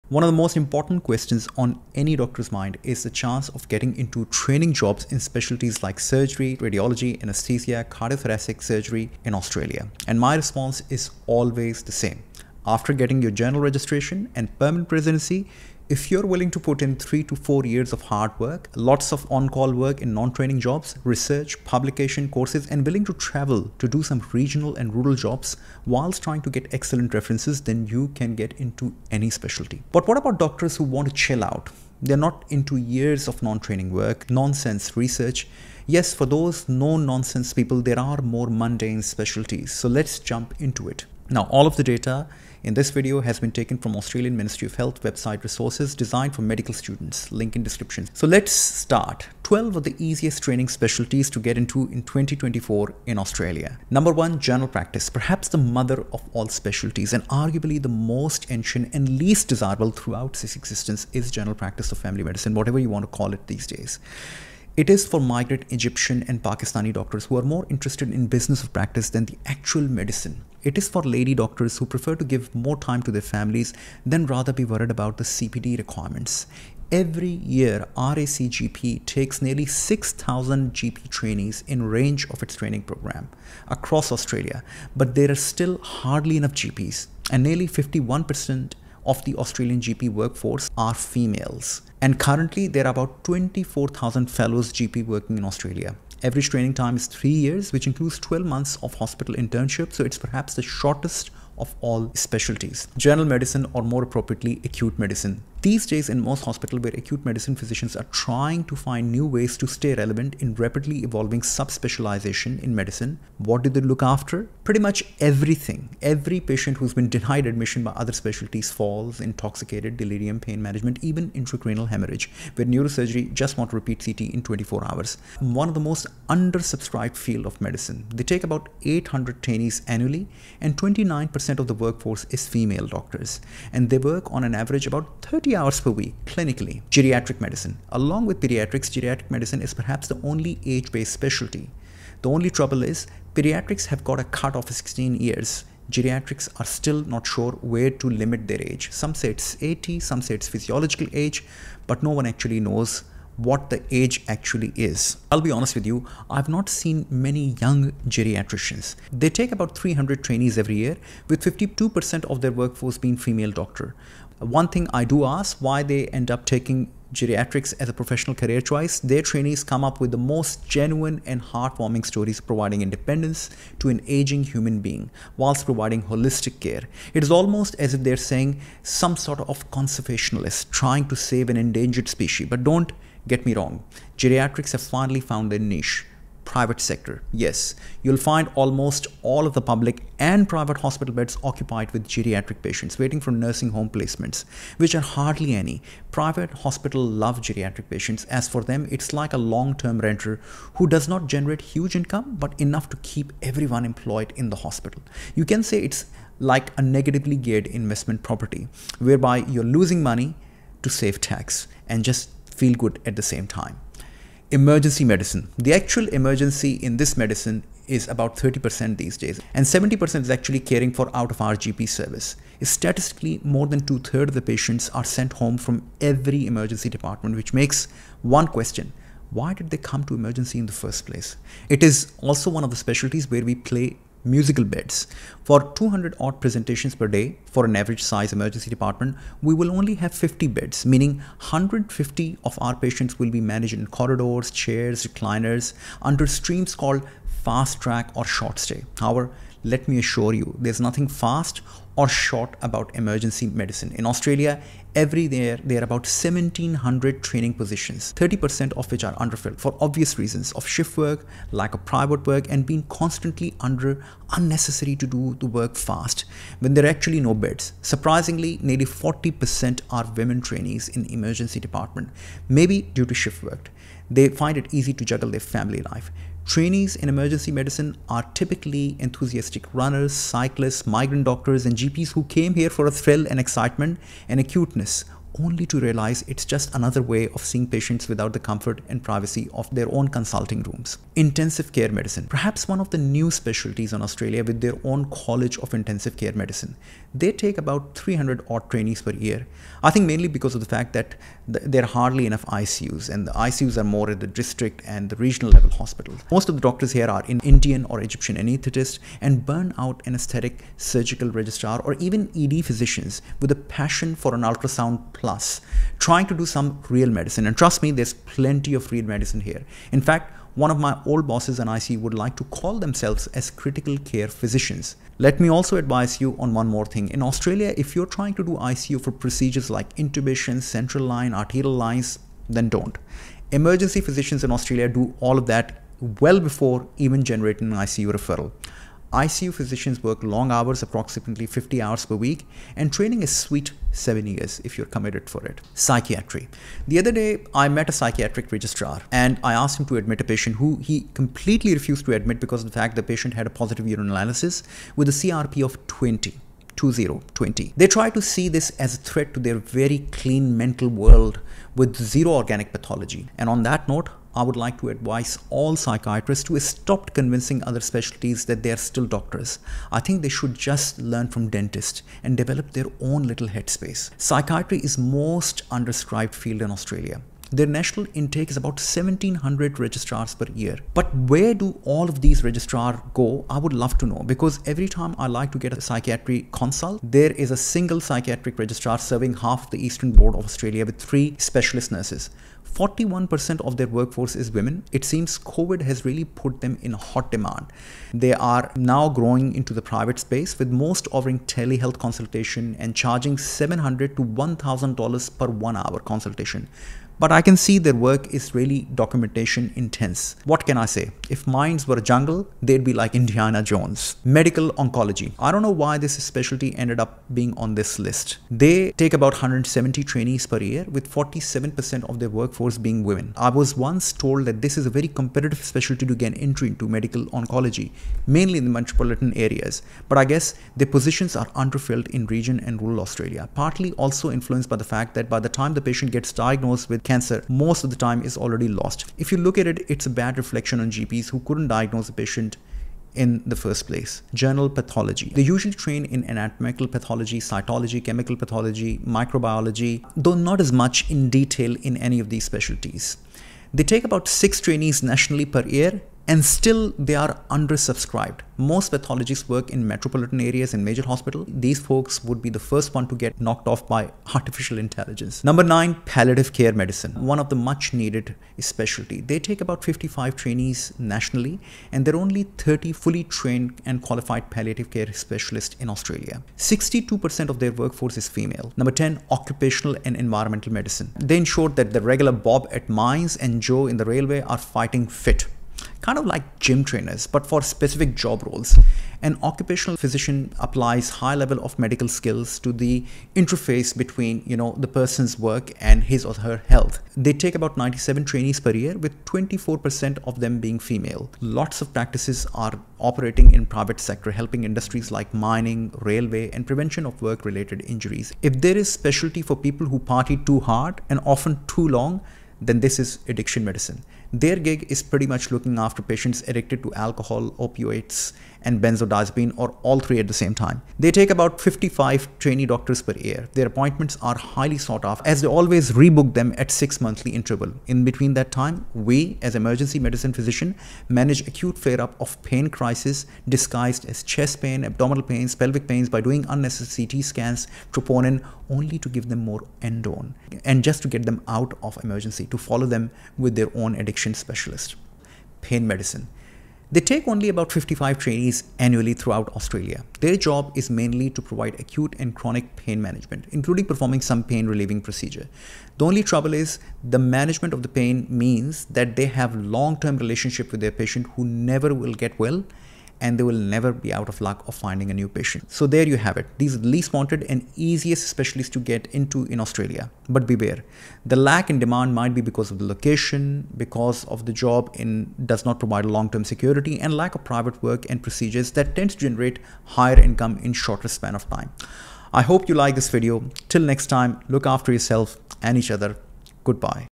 Easiest Medical Specialities for training in Australia
Easiest Medical Specialities to get training position in Australia 2024
00:00 Introduction
0:53 General Practice
1:43 General Medicine
2:18 Geriatrics
3:35 Emergency Medicine
4:50 ICU
5:47 Psychiatry
6:55 Medical Oncology
7:25 General Pathology
7:47 Palliative care
8:00 Occupational Health
8:24 Addiction Medicine
9:06 Pain Medicine